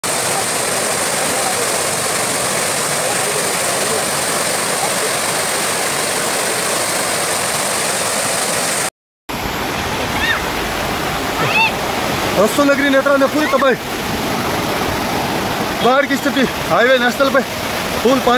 Yatra के दौरान हादसे में कई लोगों की जान🥺 ये बादल कैसे फटते है ?🥺Rajasthan
यात्रा के दौरान कई लोगों की जान, भयंकर आँधी सक्रवात तूफान 😱😱